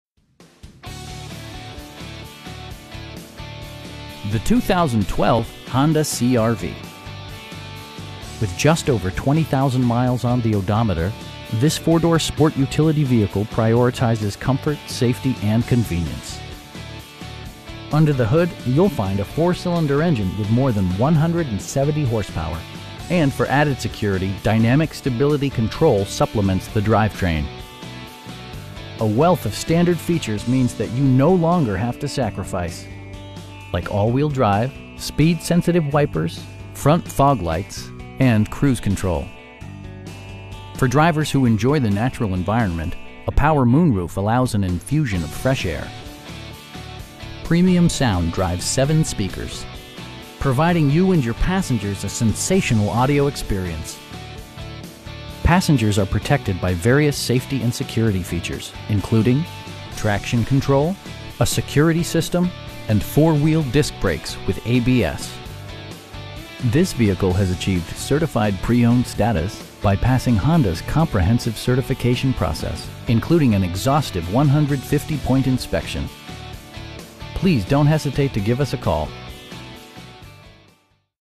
2012 Honda CR-V EX-L in Dubuque, IA 52002
Richardson Honda
1475 J.F. Kennedy Rd

The 2012 Honda CR-V. With just over 20,000 miles on the odometer, this 4 door sport utility vehicle prioritizes comfort, safety and convenience. Under the hood you'll find a 4 cylinder engine with more than 170 horsepower, and for added security, dynamic Stability Control supplements the drivetrain. A wealth of standard features means that you no longer have to sacrifice. Like all wheel drive, 1-touch window functionality, speed sensitive wipers, front fog lights, tilt steering wheel, power door mirrors and heated door mirrors, an overhead console, and cruise control. Features such as automatic climate control and leather upholstery prove that economical transportation does not need to be sparsely equipped. For drivers who enjoy the natural environment, a power moon roof allows an infusion of fresh air. Premium sound drives 7 speakers, providing you and your passengers a sensational audio experience. Passengers are protected by various safety and security features, including: head curtain airbags, front SIDE impact airbags, traction control, brake assist, a security system, and 4 wheel disc brakes with ABS. This vehicle has achieved Certified Pre-Owned status, by passing Honda's comprehensive certification process, including an exhaustive 150-point inspection! You will have a pleasant shopping experience that is fun, informative, and never high pressured. Please don't hesitate to give us a call.